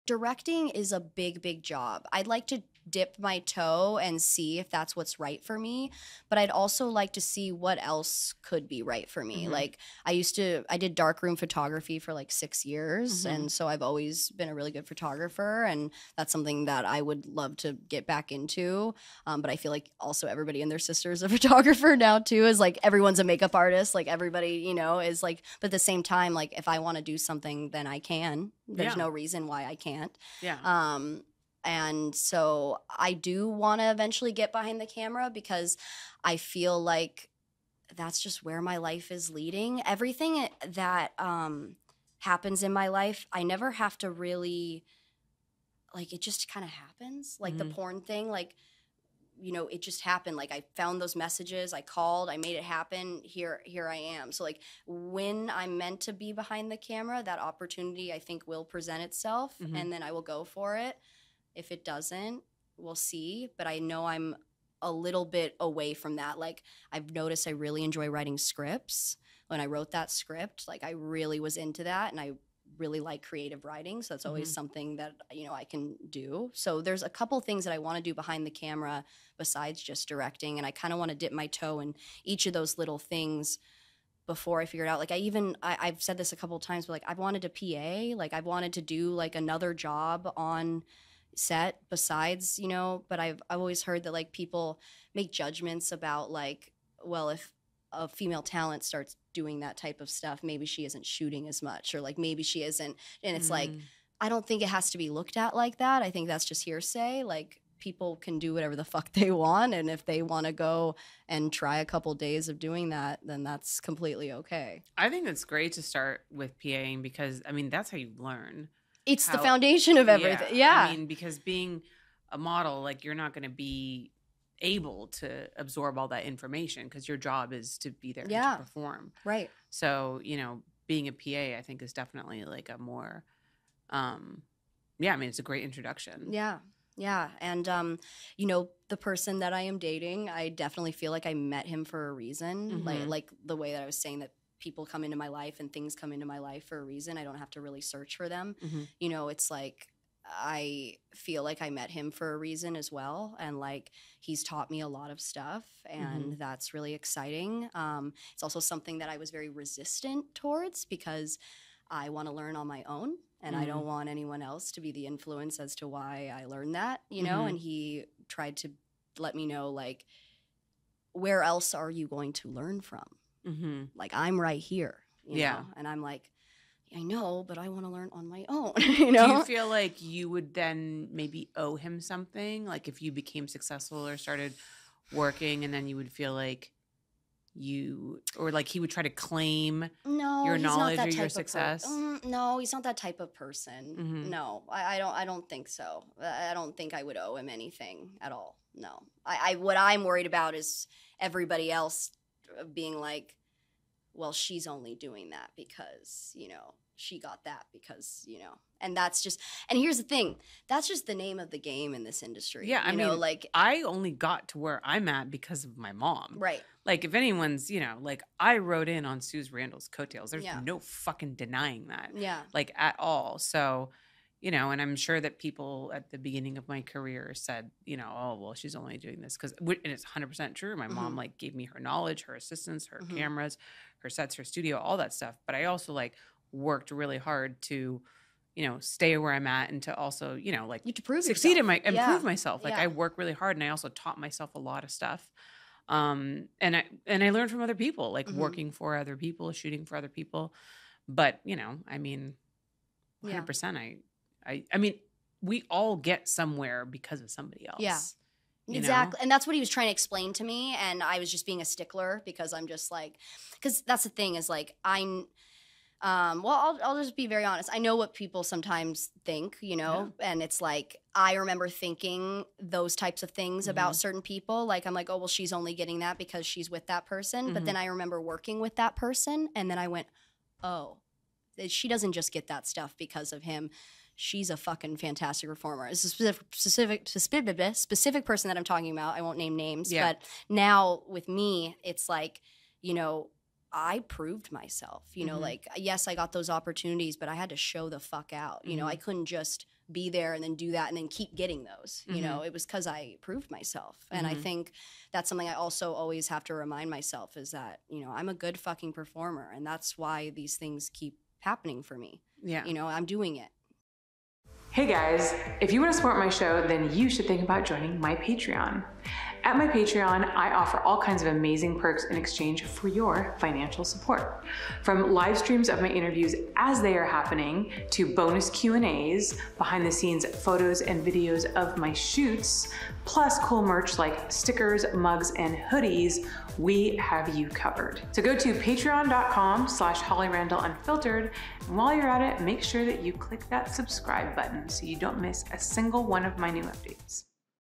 Lilly Bell Wants to Get Behind the Camera 🎥
Whether it’s directing or PAing, Lilly Bell just wants to explore her options!